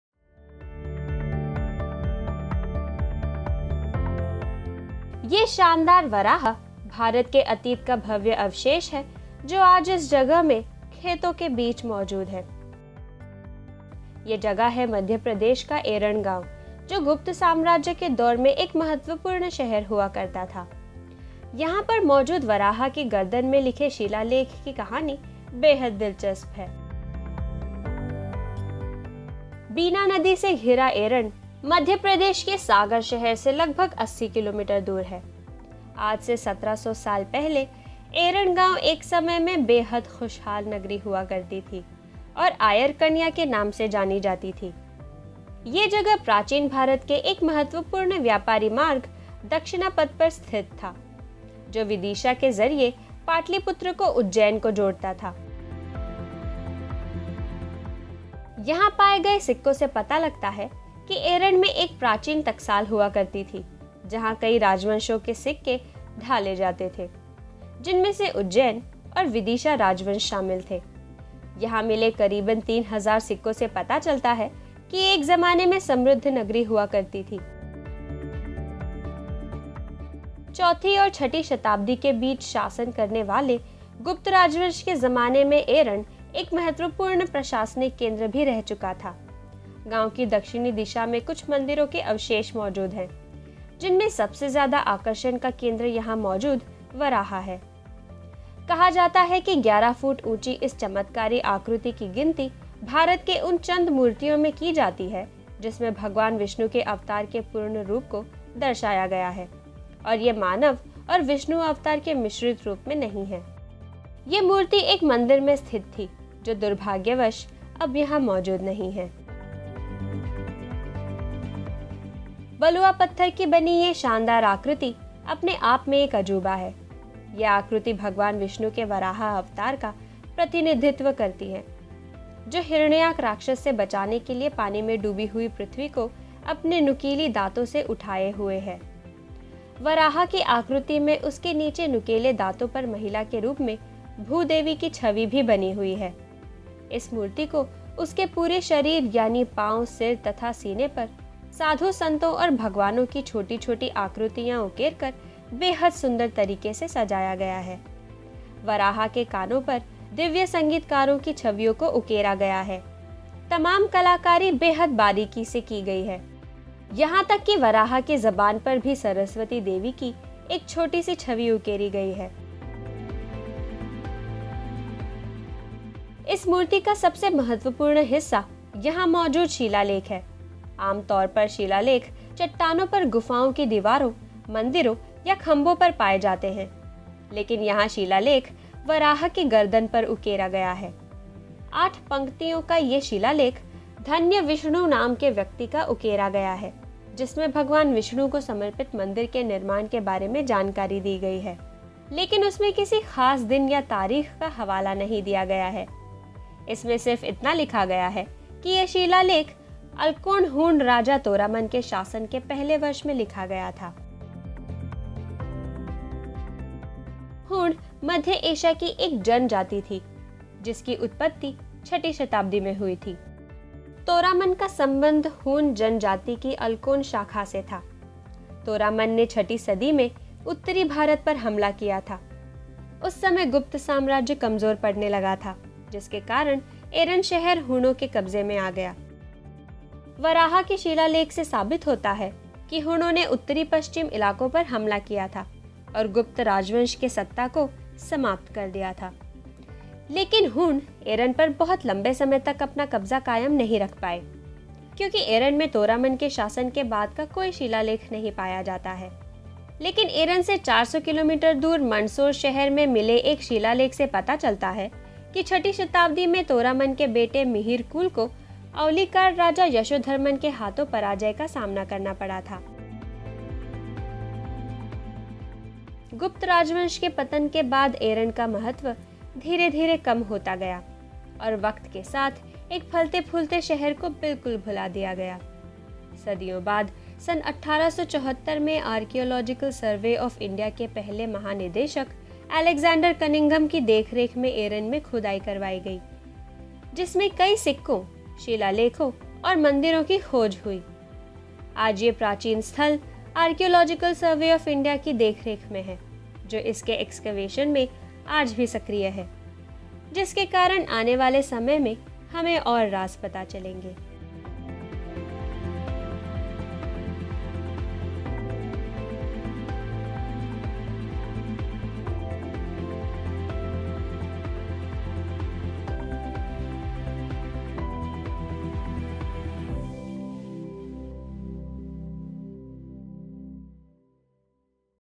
एरण मंदिर- एक समृद्ध नगरी की अनजानी दास्ताँ | Eran Temple - The Unknown Story of a Prosperous City
यह शानदार वराह, भारत के अतीत के भव्य अवशेष हैं। आज यह, इस जगह, मैदानों के बीच खड़े हैं। यह जगह यानी
मध्यप्रदेश का एरण शहर, जो गुप्त साम्राज्य के दौर में एक बहुत महत्वपूर्ण शहर हुआ करता था। और इससे भी दिलचस्प वरहा की गर्दन पर लिखे गये शिला-लेख हैं। इन शिला-लेखों की एक दिलचस्प कहानी है। बीना नदी से, तीन तरफ़ से घिरा एरण, मध्यप्रदेश के सागर शहर से 80 किलोमीटर दूर है। हालांकि आज यह जगह खंडहर में बदल चुकी है। लेकिन आज से सतरह सौ साल पहले यह जगह एक बेहद ख़ुशहाल स्थल हुआ करती थी और एराकन्या के नाम से जानी जाती थी।यह जगह, प्राचीन युग के एक महत्वपूर्ण व्यापारी मार्ग दक्षिणापथ पर स्थित थी जो विदिशा के ज़रिये पाटलीपुत्र और मथुरा को जोड़ता था।
वहां पाये गये सिक्कों से पता लगता है कि एरण में सबसे प्राचीन टकसाल हुआ करता थी. जहां कई राजवंशों के सिक्के
ढ़ाले जाते थे, जिनमें उज्जैन,विदिशा और त्रिपुरा राजवंशों शामिल थे।यह कितनी बडी बात है कि यहां,300 ईसा पूर्व से
सन 100 तक के तीन हज़ार सिक्के मिले हैं। इस बात पर भी आश्चर्य नहीं होना चाहिये कि चौथी और छठी शताब्दी के बीच,शासन करनेवाले, सबसे ज़्यादा ताक़तवर,गुप्त राजवंश के ज़माने में यह एक महत्वपूर्ण प्रशासनिक केंद्र भी रह चुका था। शहर की दक्षिणी दिशा में चंद मंदिरों के अवशेष मौजूद हैं।जिनमें सबसे ज़्यादा आकर्षण का केंद्र यहां मौजूद वराह है।कहा जाता है कि 11फ़ुट ऊंची यह चमत्कारी आकृति, देश में पायी जाने वाली अपनी तरह की पहली आकृति थी । यहां एक मंदिर में स्थित थी,लेकिन अफ़सोस की अब वह मंदिर वहां मौजूद नहीं है।

बल्युआ पत्थर की बनी यह शानदार आकृति अपने आप में एक अजूबा है।यह आकृति भग्वान विष्णु के वराह अवतार का
प्रतिनिधित्व करती है जो हिरण्यकश्यप राक्षस से बचाने के लिये पानी में डूबी हुई पृथ्वी को अपने नुकीले दोतों से उठाये हुये है। विराह की आकृति में उसके एक नुकीले दांत पर महिला के रूप में भूदेवी की छवि भी बनी हुई है। इस मूर्ती को, उसके पूरे शरीर यानी पांव,सिर, तथा सीने पर, साधू-संतों और भगवानों की छोटी छोटी आकृतियां उकेर कर, बेहद सुंदर
तरीक़े से सजाया गया है। प्रचिता के कानों पर दिव्य-संगीतकारों की छवियों को उकेरा गया है। तमाम कलाकारी बेहद
बारीकी से की गई है, यहां तक कि वराह की ज़बान पर भी सरस्वती देवी कीएक छोटी-सी छवि उकेरी गई है।

इस मूर्ती का सबसे दिलचस्प हिस्सा इस पर लिखे शिला-लेख हैं। आमतौर पर इस तरह के शिला-लेख, चट्टानों पर गुफाओं की दीवारों, मंदिरों या खम्बों पर पाये जाते हैं,लेकिन यहां शिला-लेख वराह की गर्दन पर उकेरे गये हैं। आठ पंक्तियों का एक शिला-लेख धन्यविष्णु नाम के व्यक्ति का उकेरा गया है, जिसमें भगवान विष्णु को समर्पित मंदिर के, निर्माण के बारे में जानकारी दी गई है। लेकिन उस में किसी खास दिन या तारीख़ का हवाला नहीं दिया गया है।इसमें सिर्फ़ इतना लिखा गया है कि यह शिला-लेख, अलचोन हूण राजा तोरामन के शासन के पहले वर्ष में लिखा गया था। बताया जाता है कि तोरामन ने छठी सदी में राज किया था।

अब सवाल यह कि हूण कौन थे ? हूण दरअसल मध्य एशिया की एक जन-जाति थी जिसकी उत्पत्ति छठी शताब्दी में
हुई थी।तोरामन, का सम्बंध हूण जन-जाति की अलचोन शाखा से था। तोरामन ने छठी सदी में,उत्तरी भारत पर हमला
किया था।उस समय गुप्त साम्राज्य कमज़ार पड़ने लगा था। इसी लिये एरण शहर आसानी से हूणों के क़ब्ज़े में आ गया।
वराह के शिला-लेख से साबित होता है कि हूणों ने उत्तरी-पश्चिमी इलाक़ों पर हमला किया था और गुप्त राजवंश के
की सत्ता को समाप्त कर दिया था। लेकिन हूण, एरण पर बहुत लम्बे समय तक अपना क़ब्जा क़ायम नहीं रख
पाये।क्योंकि एरण में तोरामन के शासन के बाद का कोई शिला-लेख नहीं पाया जाता। लेकिन एरण से 400 किलोमीटर
दूर मंदसौर शहर में मिले एक शिला-लेख से पता चलता है कि छठी शताब्दी में, तोरामन के बेटे मिहिरकुल को
औलिकार राजा यशोवर्धन के हाथों परजय का सामना करना पड़ा था।

गुप्त राजवंश के पतन के बाद, एरण का महत्व धीरे धीरे कम होता गया। और वक़्त के साथ, एक फलते फूलते शहर
को बिल्कुल भुला दिया गया। बरसों बाद, सन 1874 में, भारतीय पुरातत्व सर्वेक्षण के पहले महानिदेशक अलेक्ज़ैंडर
कनिंगघम की देखरेख में एरण में खुदायी करवाई गई जिसमें कई सिक्कों,कई शिला-लेखों और कई मंदिरों खोज हुयी।
आज यह प्राचीन स्थल भारतीय पुरातत्व सर्वेक्षण( एएसआई) की देखरेख में है और एक ज़माने के ख़ुशहाल शहर की
निशानी के तौर पर मौजूद है। एएसआई की तरफ़ से आज भी यहां खुदाई का काम जारी है। अब यह वक़्त ही बतायेगा
कि एरण के बारे में अभी और क्या क्या राज़ छुपे हुये हैं जिनके सामने ने का इंतज़ार करना होगा।